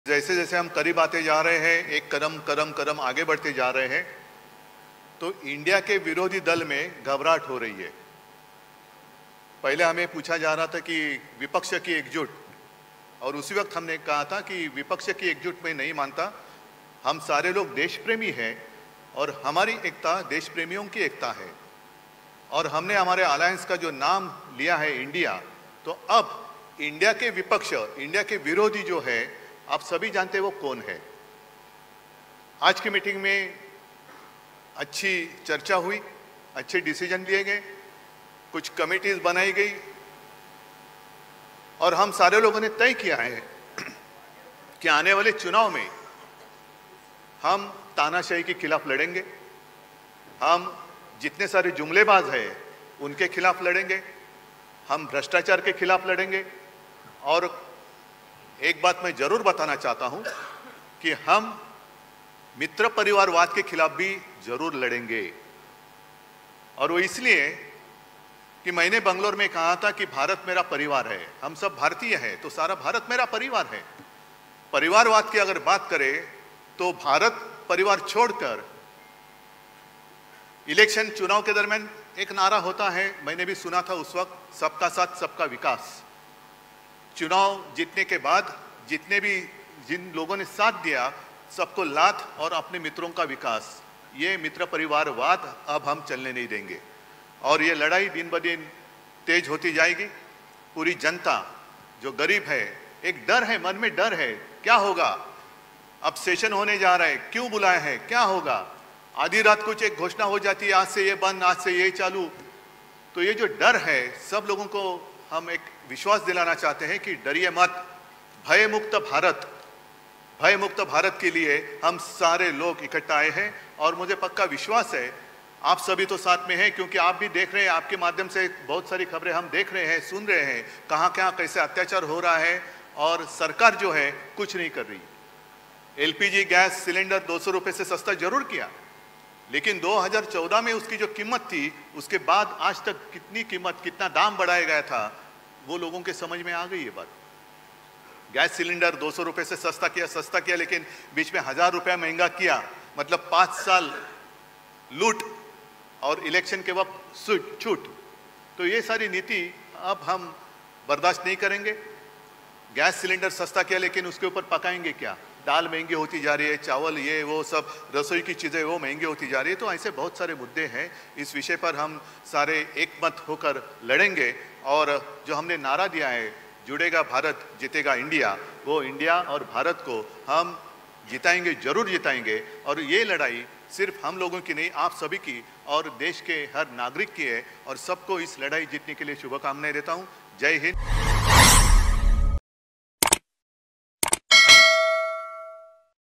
विपक्षी पार्टियों के जाल में फंस चुके हैं मोदी ? Uddhav Thackeray का INDIA गठबंधन को लेकर खुलासा
विपक्षी पार्टियों के जाल में फंस चुके हैं मोदी ?